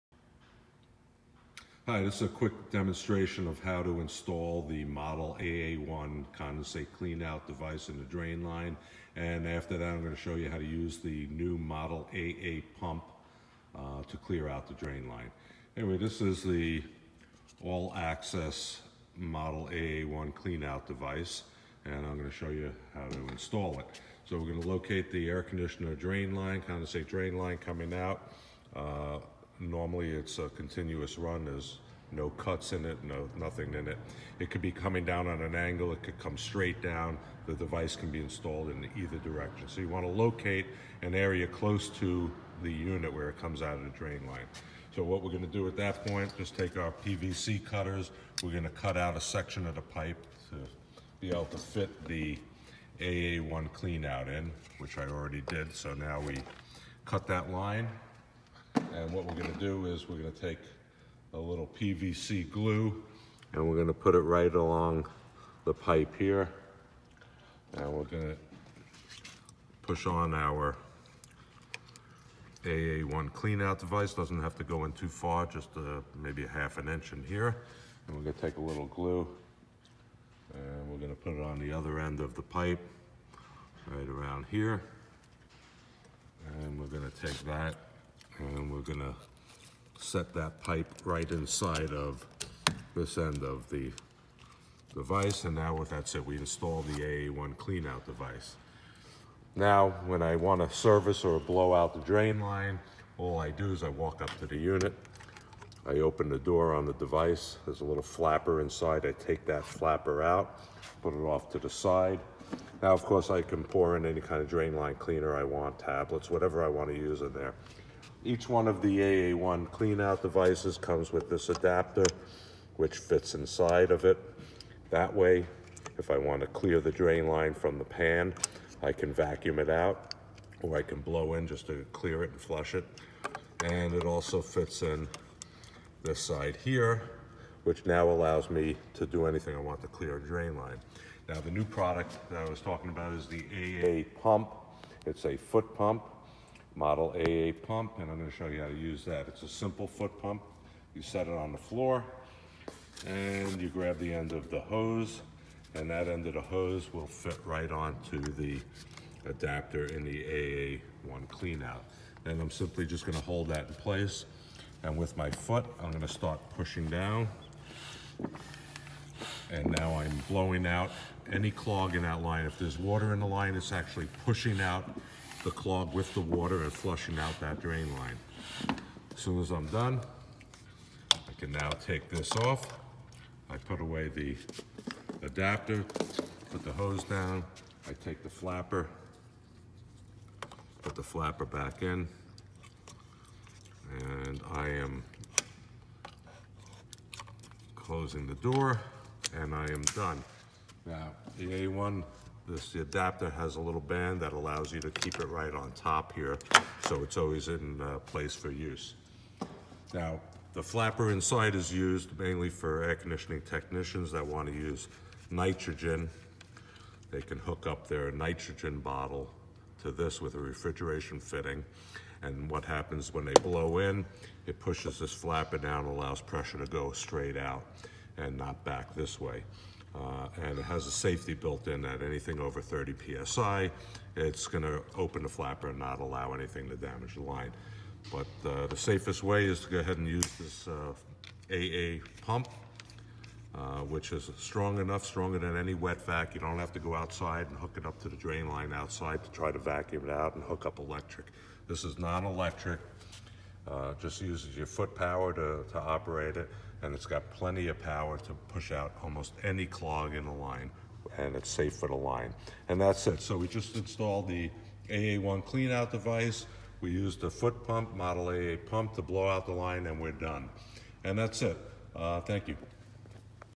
How To Install Model AA1 A/C Drain Line Cleanout Device
Install of Model AA1 A/C Drain Line Cleanout Device and Clearing The Line With The ALL-ACCESS Model AA-PUMP.
for more info & products.